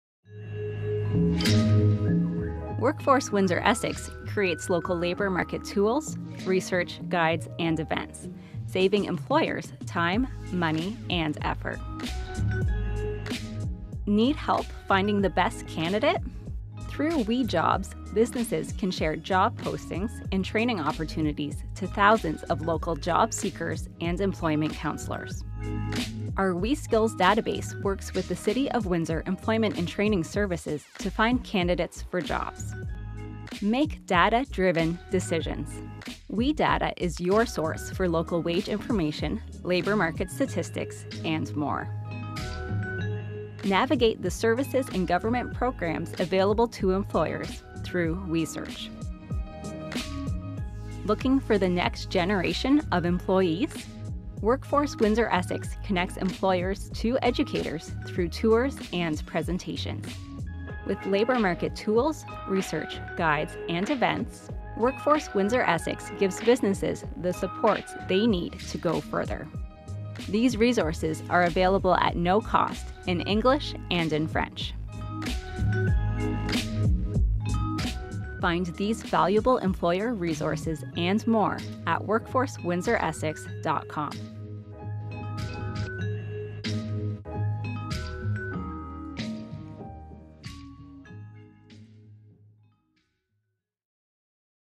Workforce WindsorEssex can Save Employers Time, Money and Effort.
Workforce WindsorEssex can help support your company's Human Resources efforts!
Learn more about how Workforce WindsorEssex can save employers time, money and effort

Need help finding the best candidate? Share your job postings and find out about upcoming career fairs by signing up for WEjobs

Make data driven decisions. Request custom labour market information, such as wages, through WEdata

Find grants, loans and services offered by government and community organizations through WEsearch

Workevents connect employers to schools and educators

Use Workforce WindsorEssex' labour market tools, research, guides and events to save you time and effort.